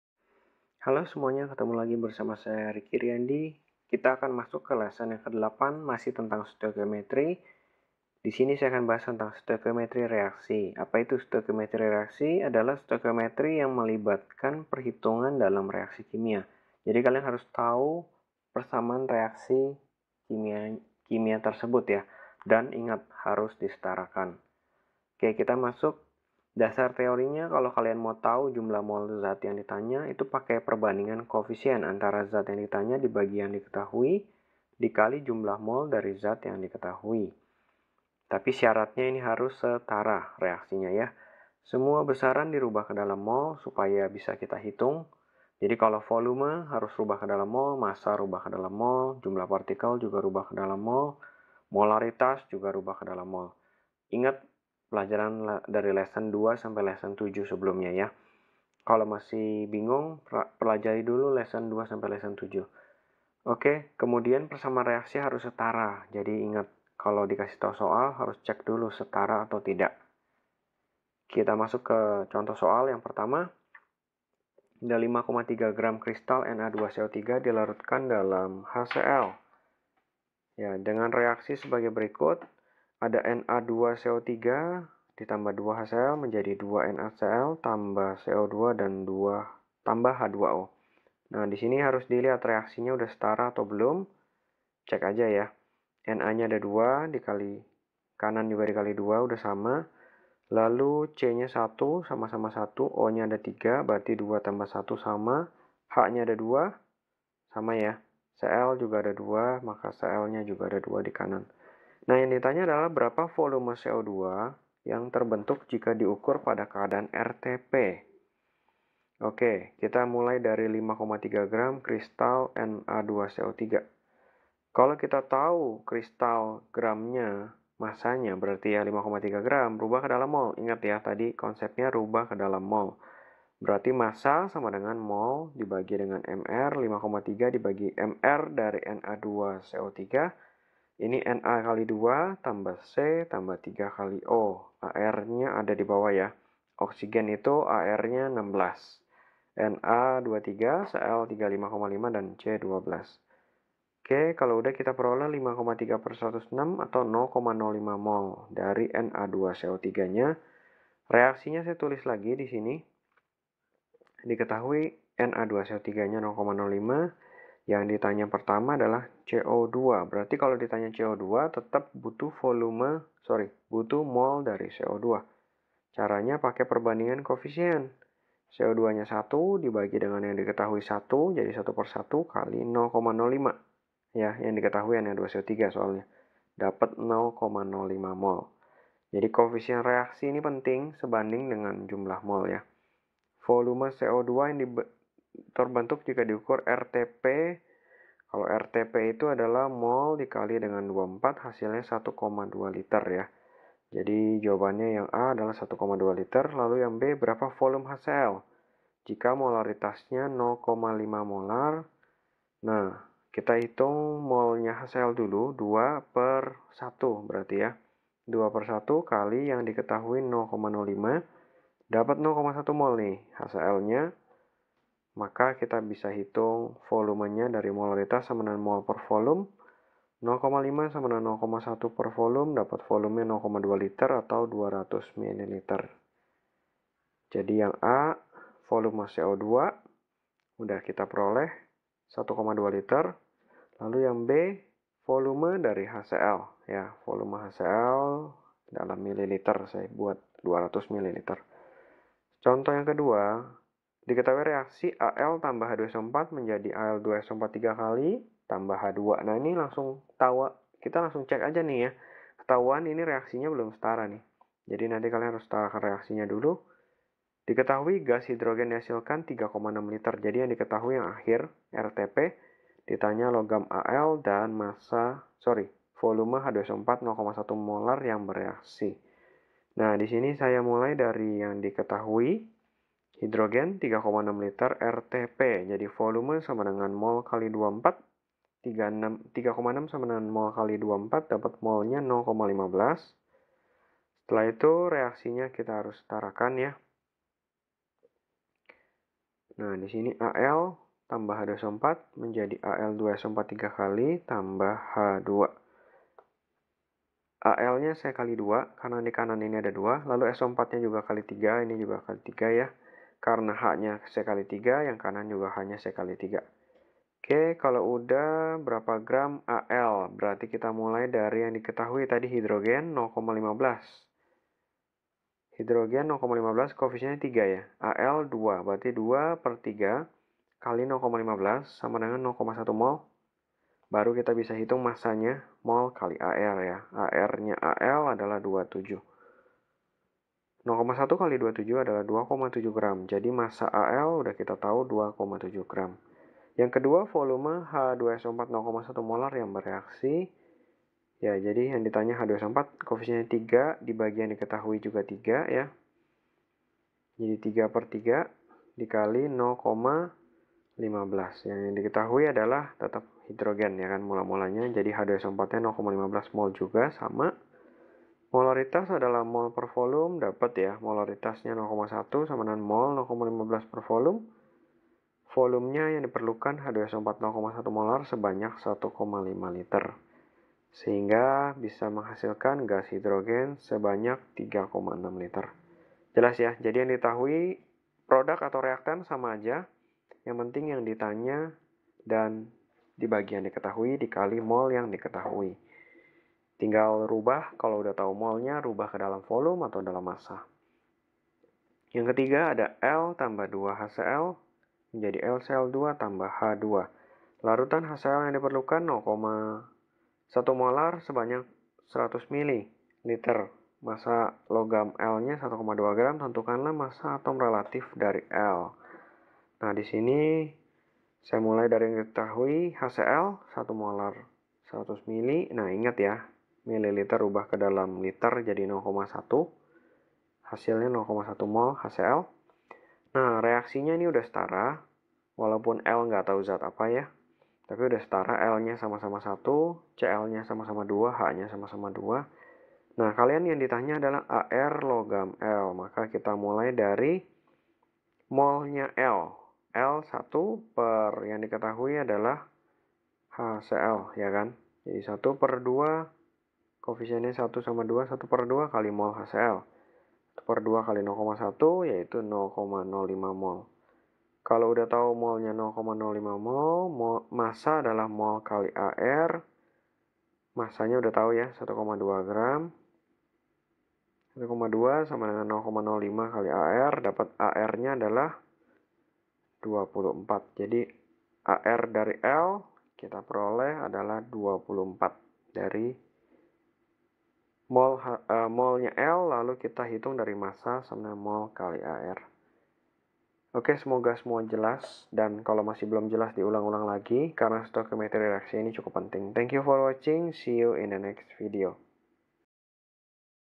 STOIKIOMETRI REAKSI (Stoikiometri Kimia - SBMPTN, SMA)
Stoikiometri reaksi melibatkan perhitungan konsep mol dalam persamaan reaksi kimia.